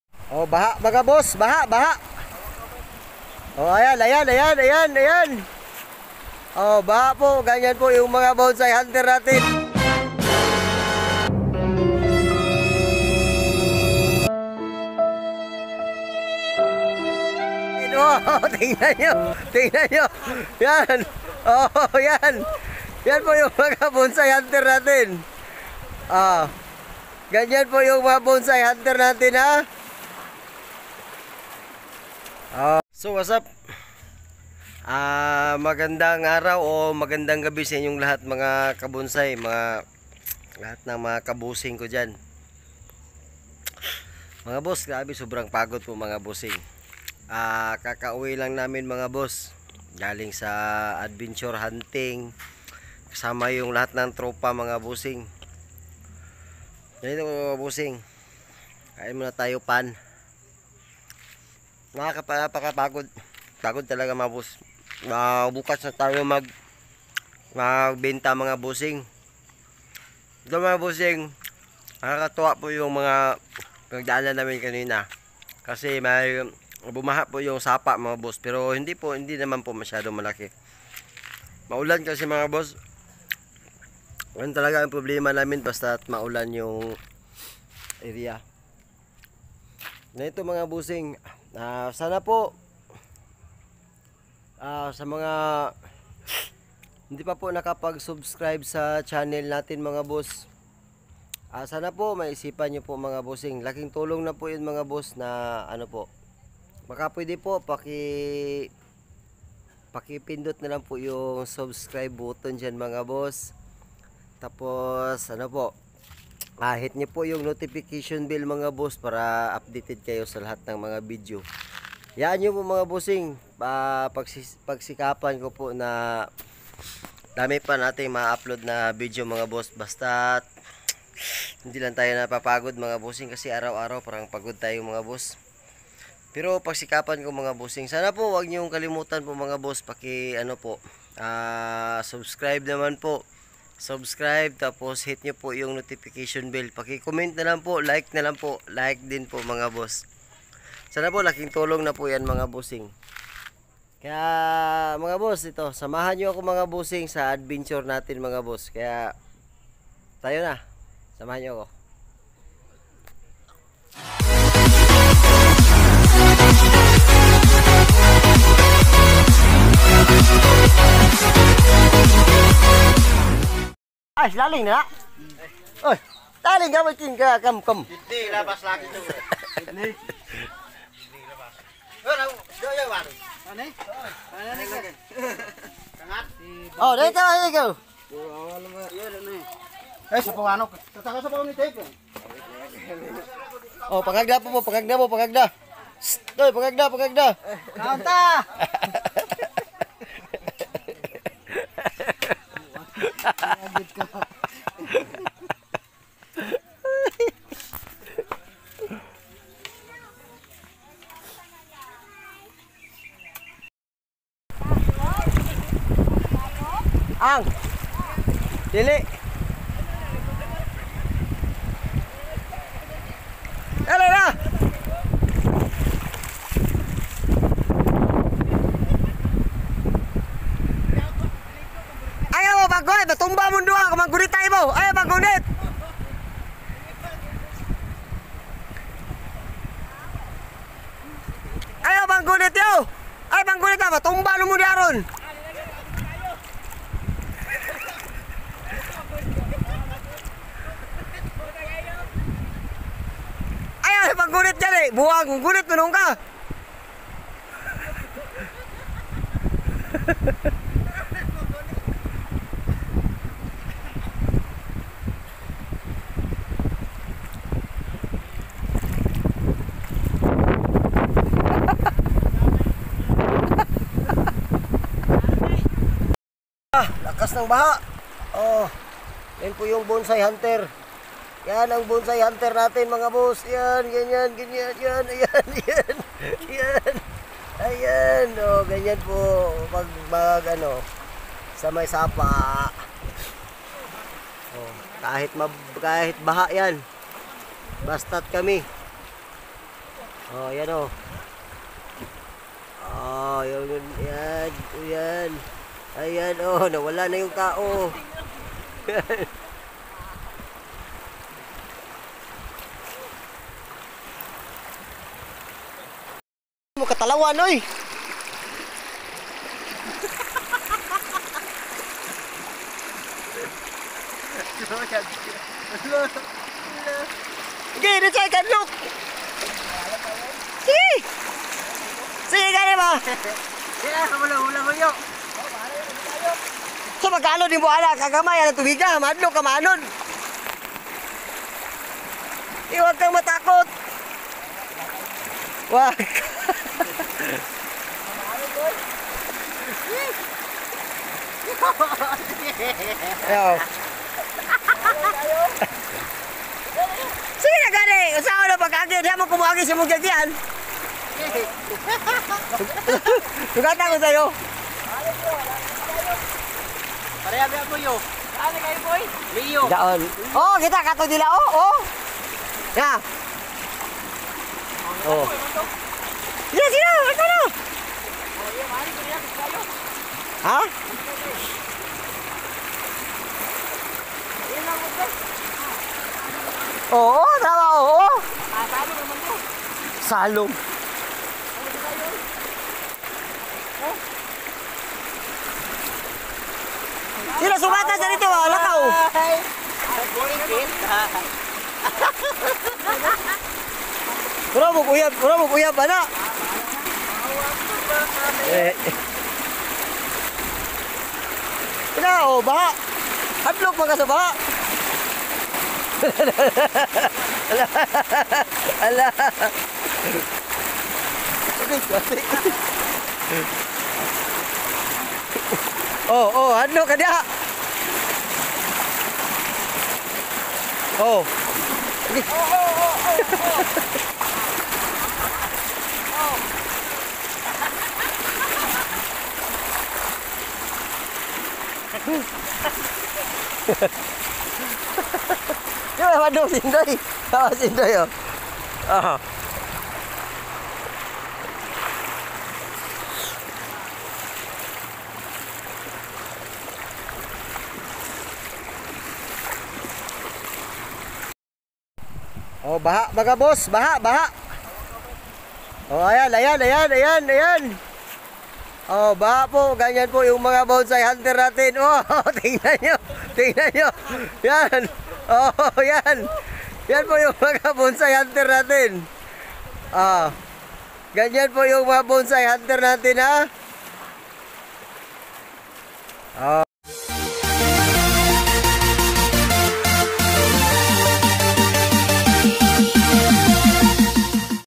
grabe ang hirap mag hunt..@charliemixvlog4554
grabi ang hirap..sa pag hunt nang bonsai materyal..